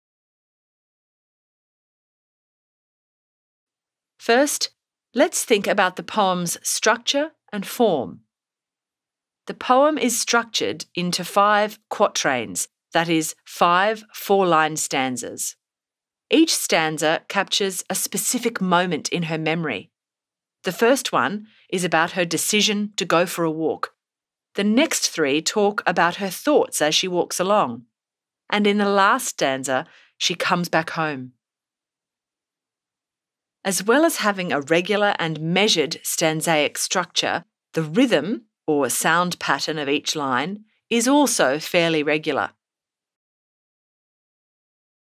Cock Crow by Rosemary Dobson (Lesson Preview 1)
📺 In this lesson's analysis, we take a look at Cock Crow by Rosemary Dobson. Watch the full lesson on Schooling Online 👇

Discover Dobson’s deeply personal and emotive reflection on the complex experience of motherhood.

Being a mum is tough. Sometimes, Dobson just wants to escape it all. One night, she decides to go on a long walk. She walks along the dark road and eventually returns home. But has she betrayed her family by wanting freedom?